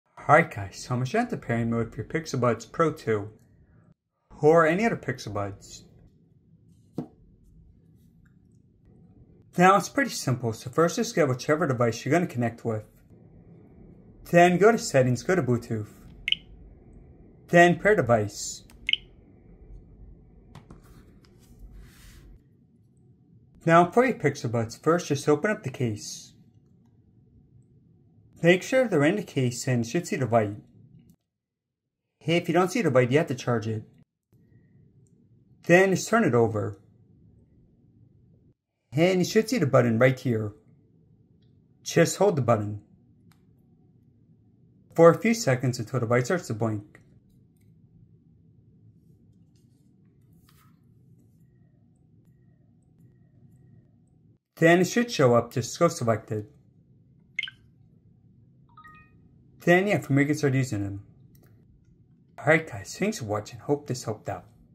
Pixel Buds Pro 2 (or ANY): How to Enter Pairing Mode!
How to enter pairing mode to connect to a Bluetooth debvice on Google Pixel Buds Pro 2 or other Pixel Buds. teampixel giftfromgoogle @madebygoogle